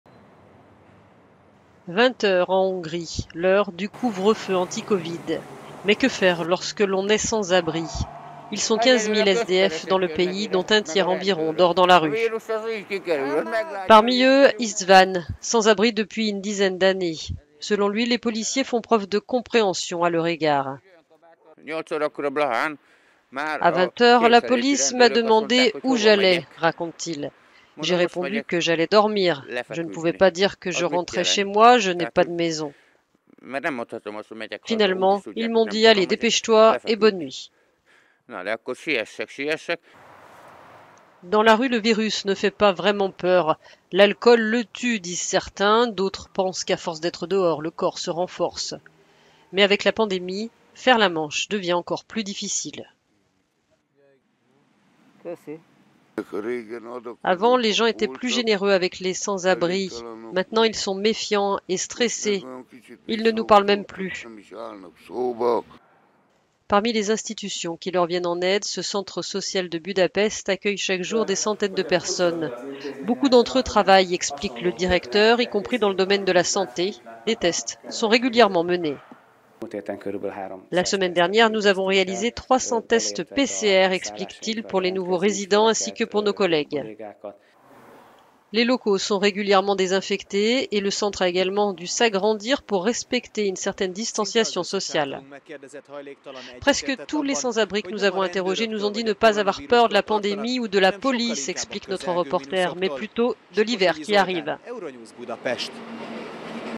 En Hongrie, les sans-abri redoutent davantage l'hiver que la Covid
La pandémie affecte aussi la vie de ceux qui vivent dans la rue. Reportage à Budapest.